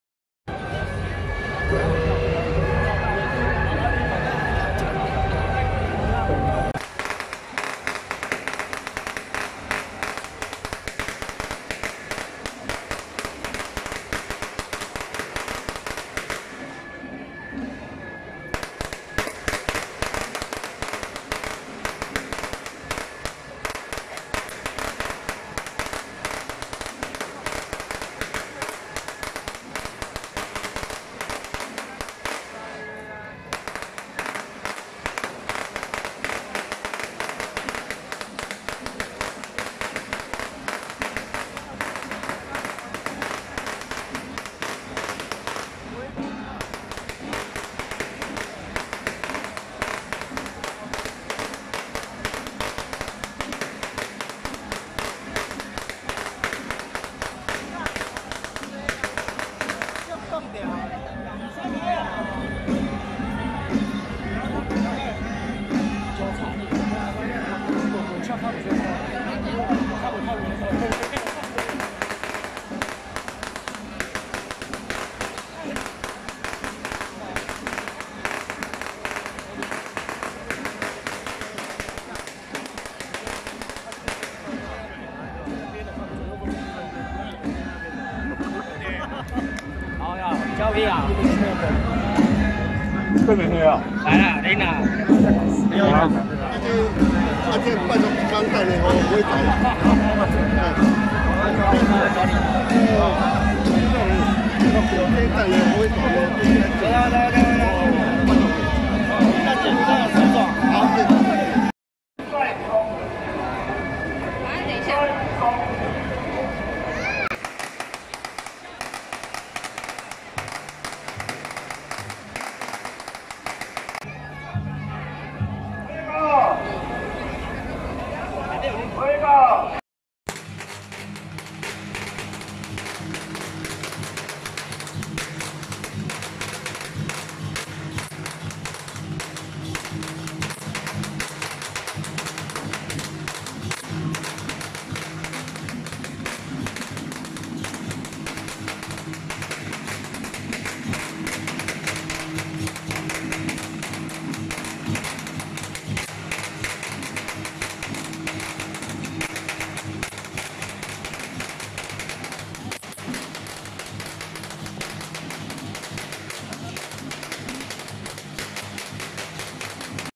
【新北建和宮-主帥轎~北港建和媽 聖駕(臺北鳳鳴堂轎班會)】~109新北建和宮北港建和媽南巡謁祖進香回駕遶境
新北建和宮-主帥轎~北港建和媽 聖駕(臺北鳳鳴堂轎班會)